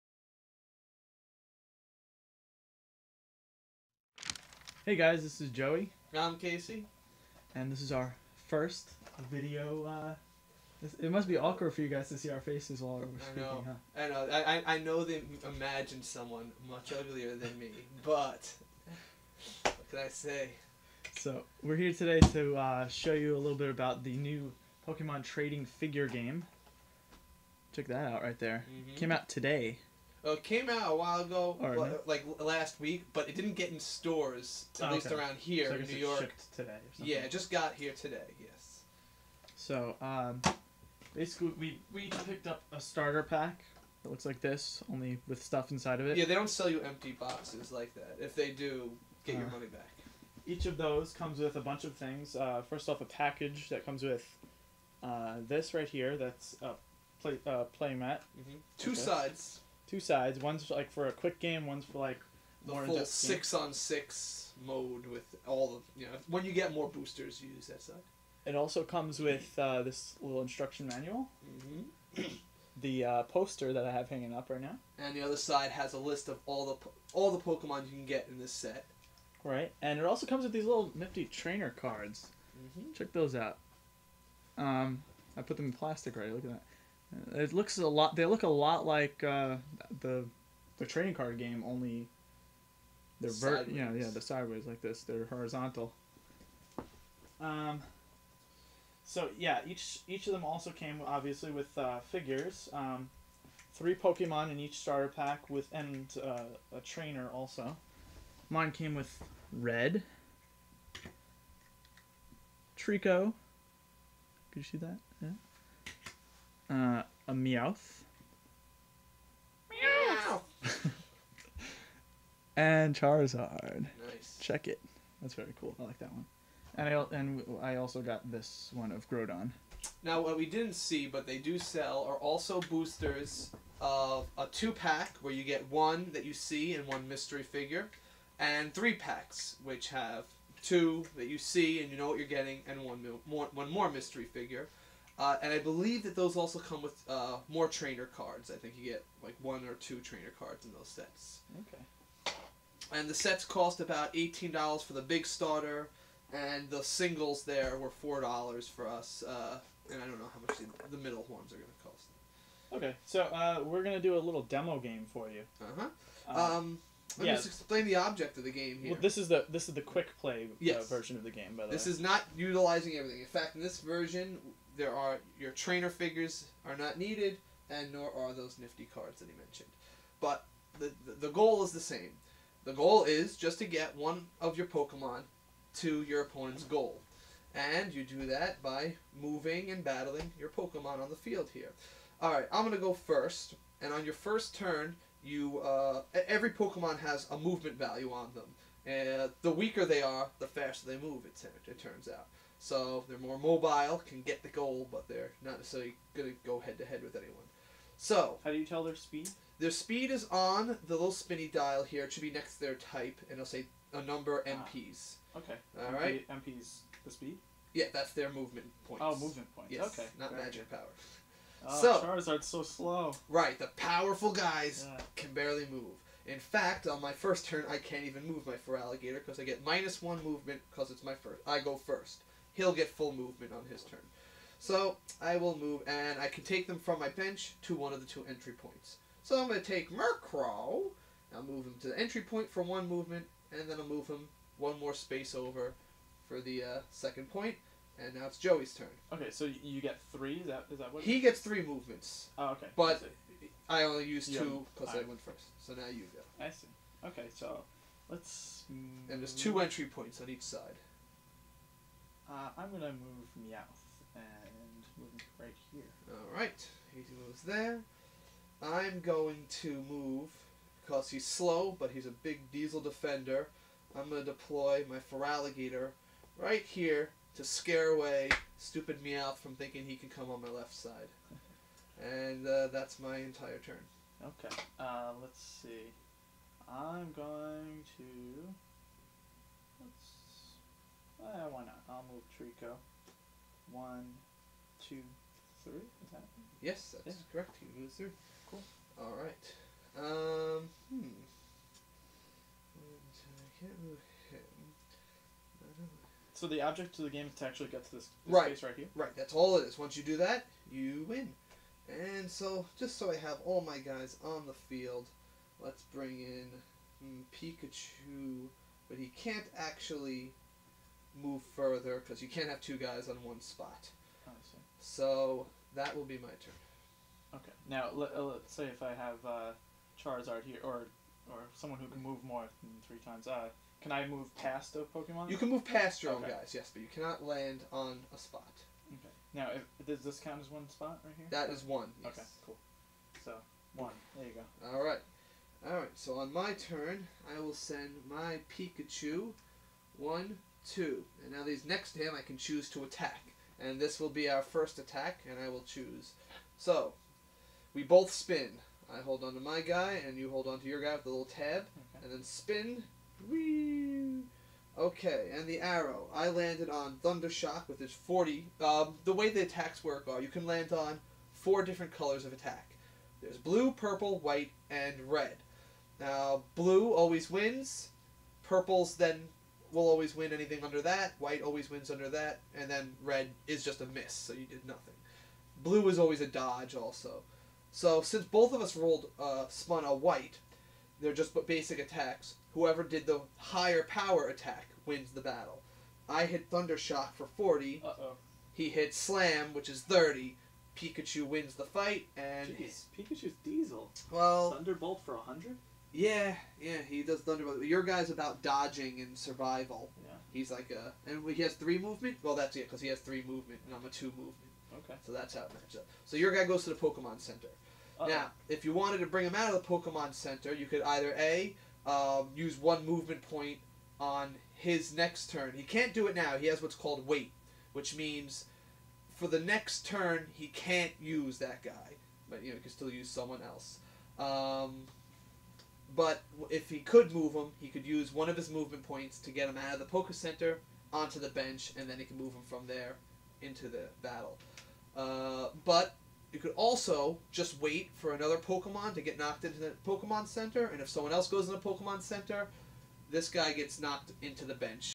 Pokemon Trading Figure Game Demo Part 1
Jowy and KC give a demo of the Pokemon Trading Figure game.